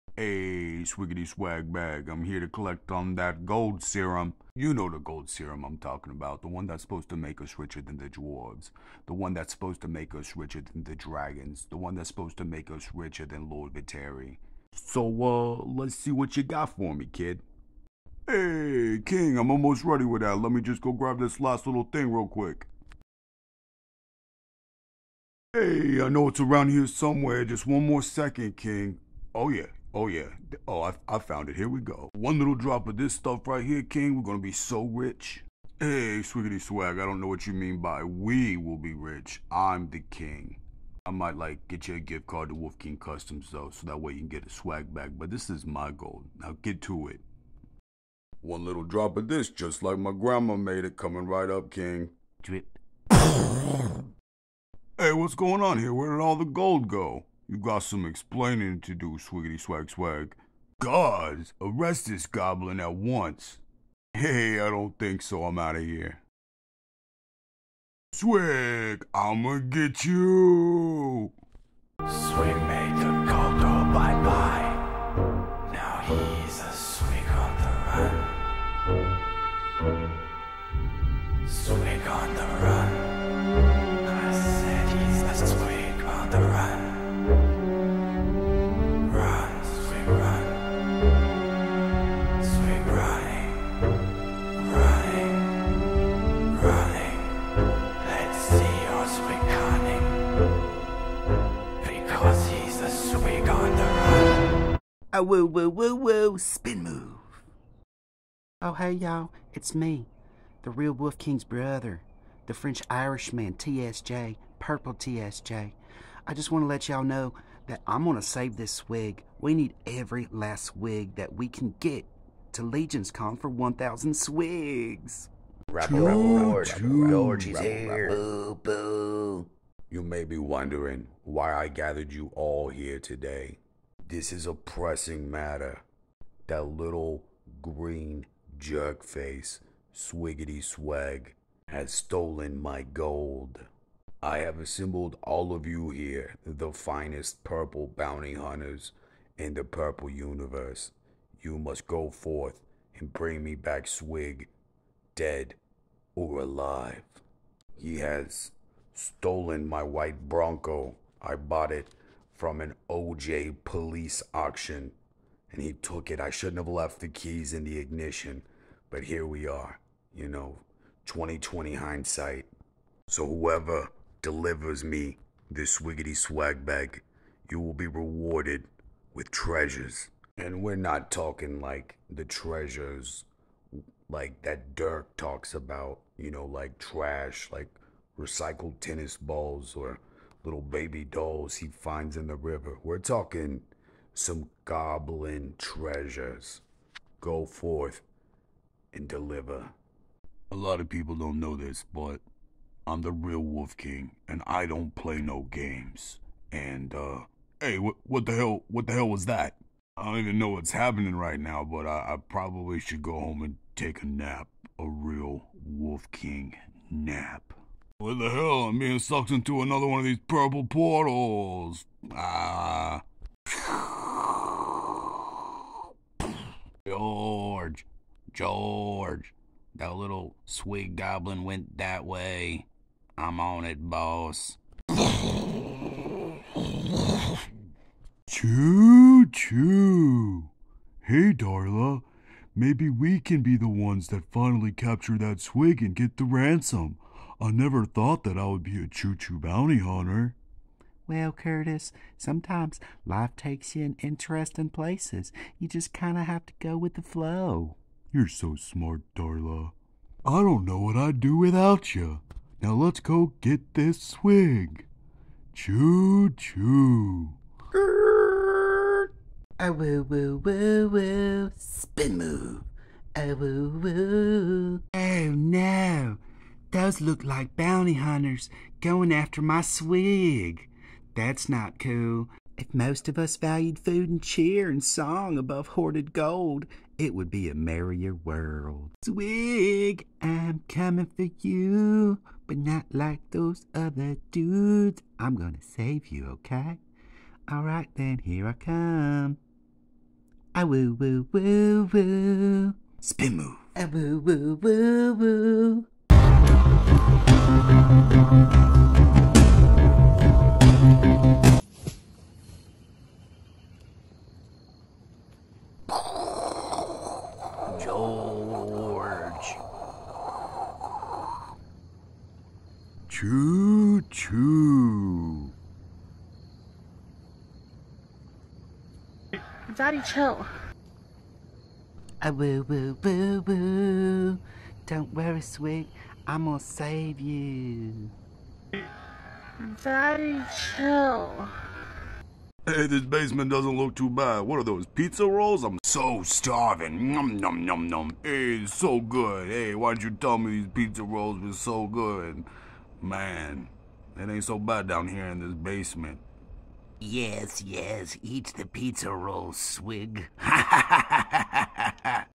Swigg on the run! mythic legions goblin shenanigans!
What caused Swigg to go on the run? What is 1000 Swiggs? Why is Legions Con going to be so amazing? This video might answer one of these questions. Enjoy!

Bounty hunters? Purple? King Nog'lin? Swigg? Swiggety Swag Bag???